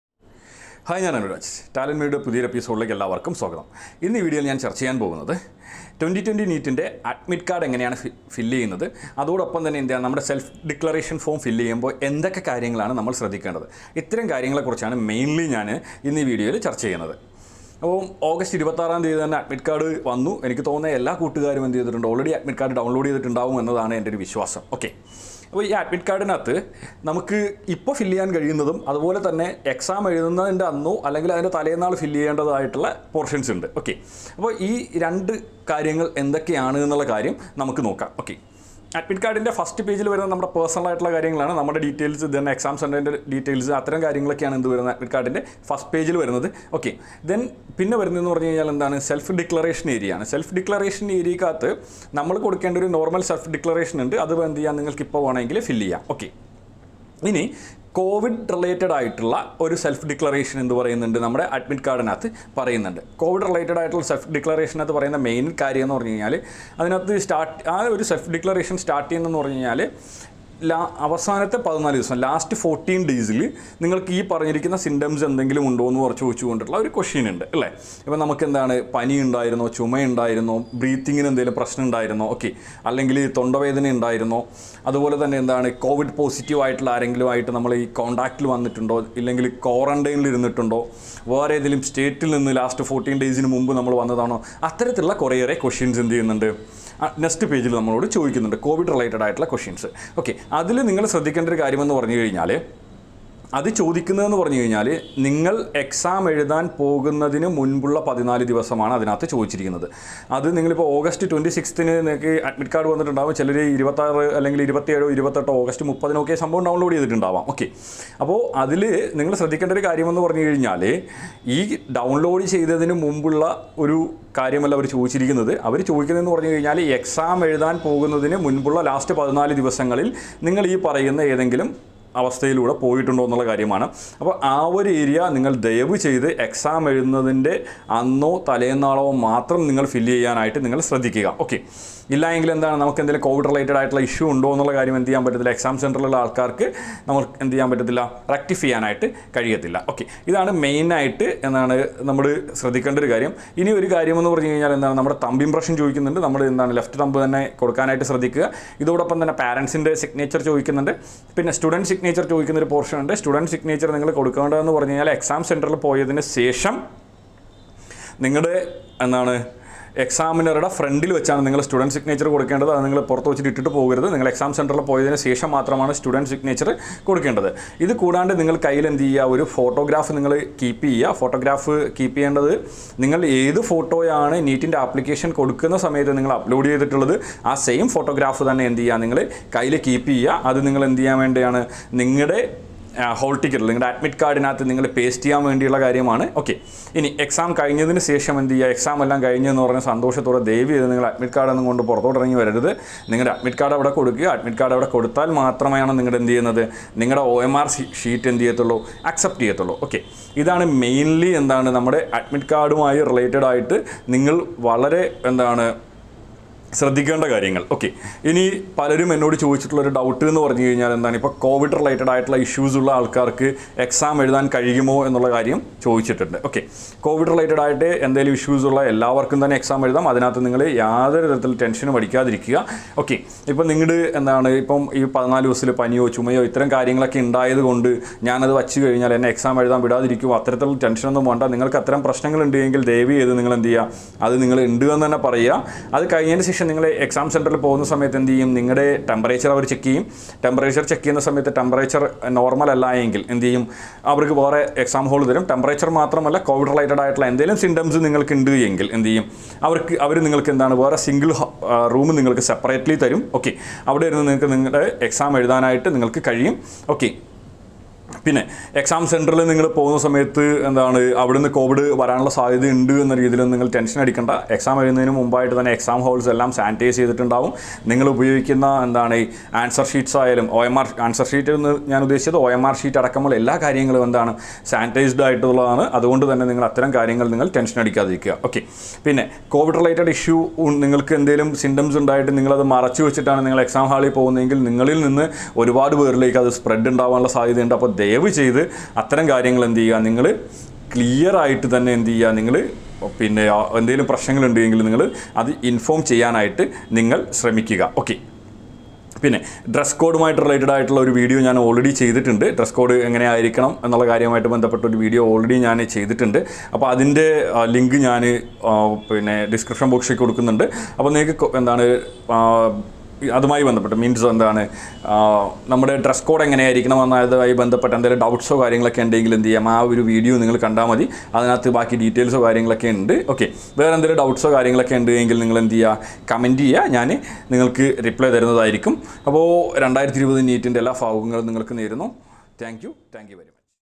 NEET Admit card വന്നു|അതിലെ പ്രധാനപ്പെട്ട കുറച്ച് കാര്യങ്ങൾ|How to fill Admit Cad| Self Deceleration
This Video about how to fill NEET Self Declaration Form ?
How to Fill NEET Self Declaration Form in Malayalam | NEET Admit Card | NEET Admit card filling.

NEET 2020 Dress Code| നിങ്ങൾ അറിഞ്ഞിരിക്കേണ്ടത്| Dress code for Boys and Girls

Mr. Anu Raj Rajamony